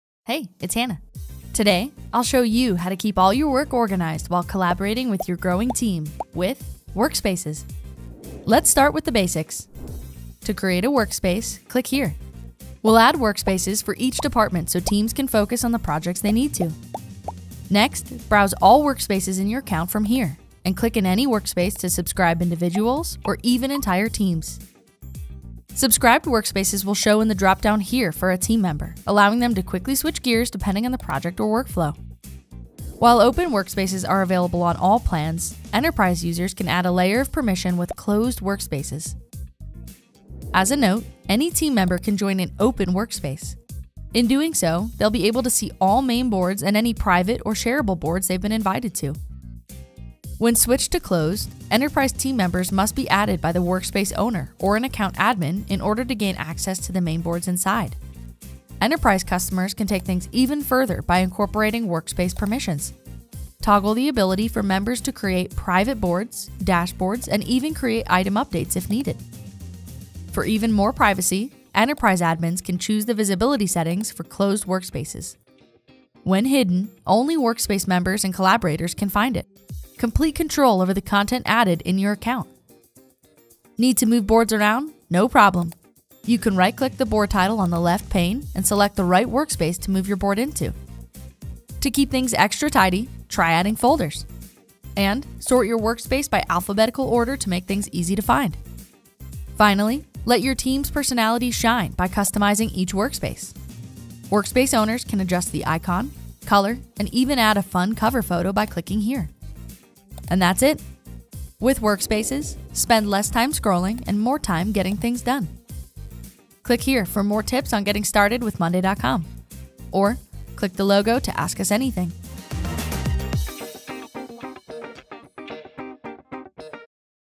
Workspaces | monday.com tutorials
With workspaces on monday.com, spend less time scrolling and more time getting things done! Hannah shows you how to configure workspaces to keep your team laser focused on the projects relevant to them.

00:00 Intro
00:08 Create a workspace
00:17 Browse // switch between workspaces, subscribe users and teams
00:32 Open, closed, and hidden workspaces
01:22 Move boards between workspaces
01:30 Add folders and sort workspace
01:39 Customize workspace
01:50 Outro

Want to deep dive into this topic? Check out our Academy and more resources in our help center!